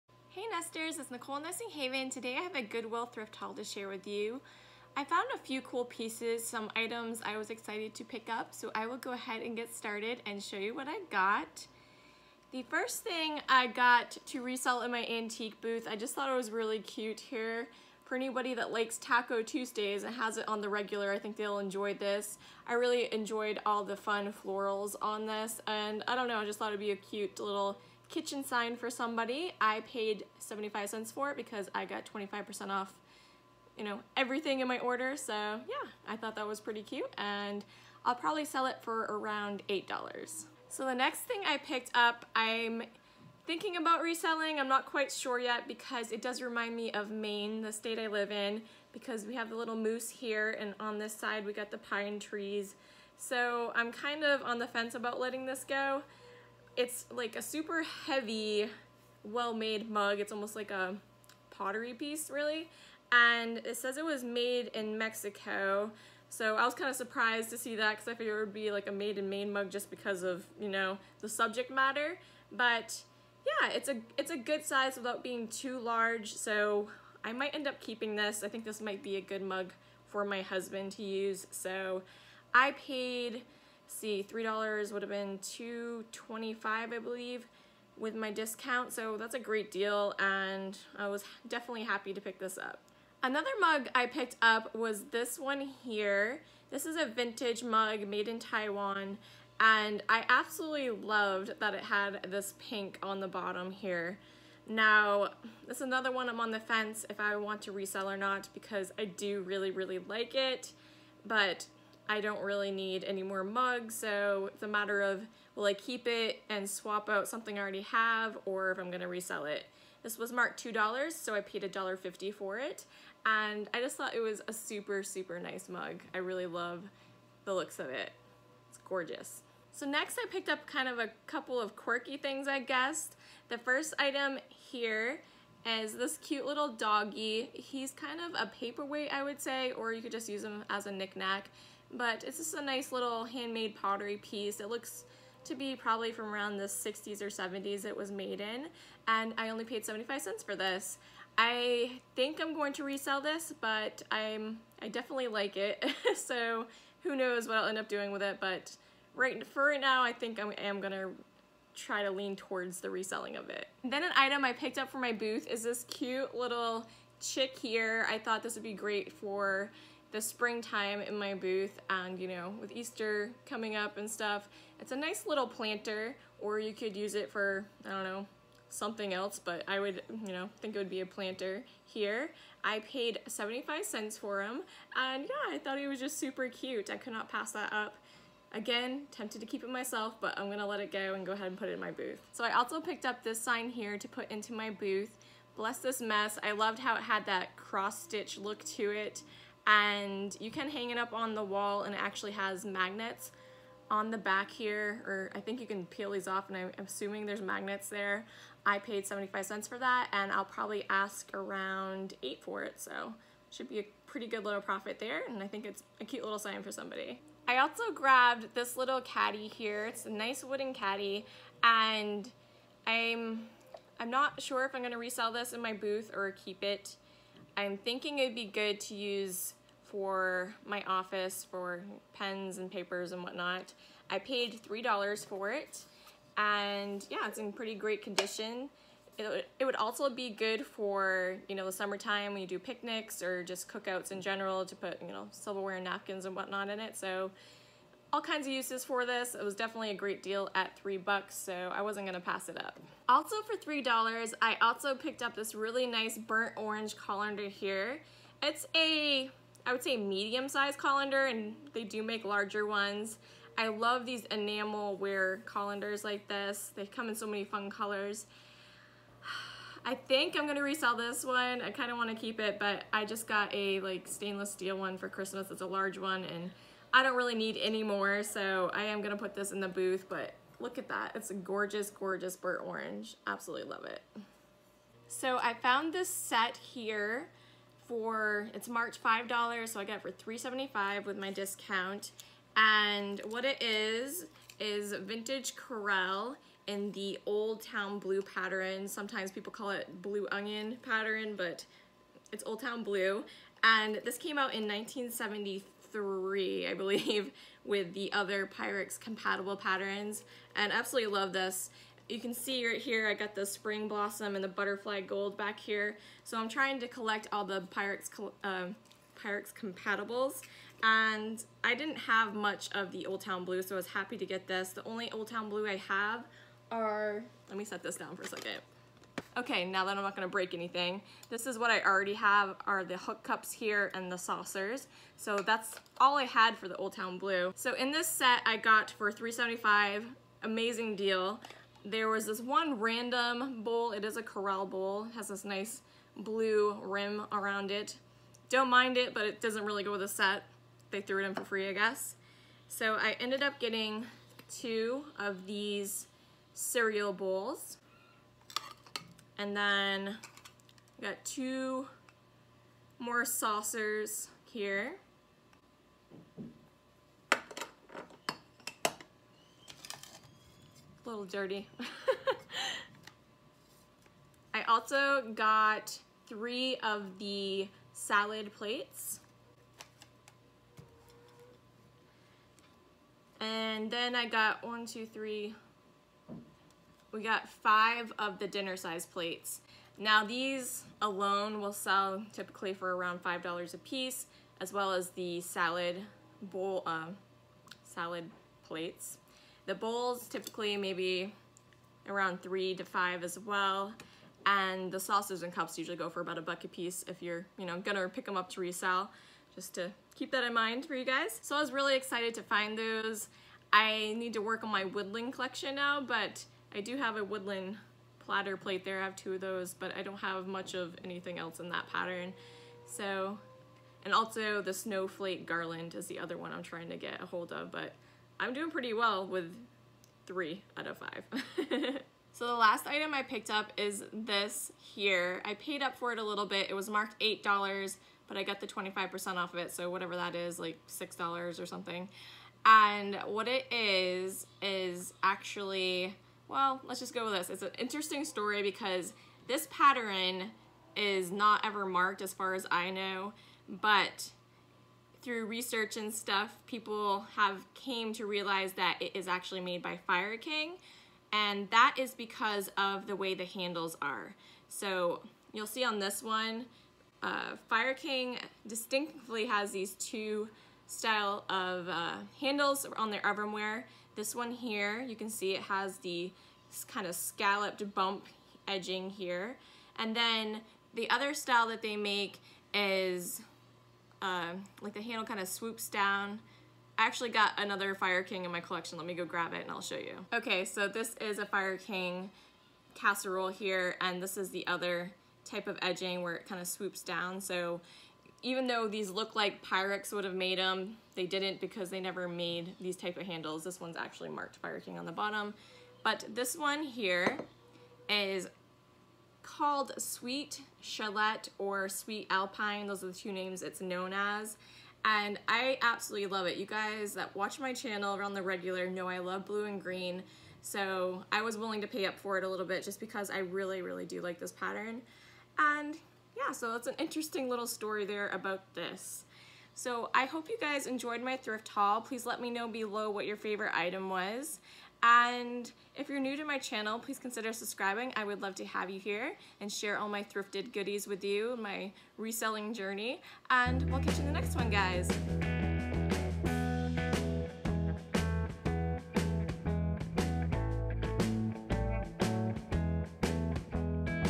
Goodwill Thrift Haul | Vintage Corelle, Unmarked Fire King + Other Fun Finds!
Hi Nesters! Today I am sharing with you a Goodwill Thrift Haul. In this thrift haul I got an AMAZING deal on some Vintage Pyrex Compatible Corelle. I also Thrifted an absolutely beautfiul unmarked vintage fire king casserole! You guys know I love thrifting for my blue and green vintage glassware! This piece is perfect with them!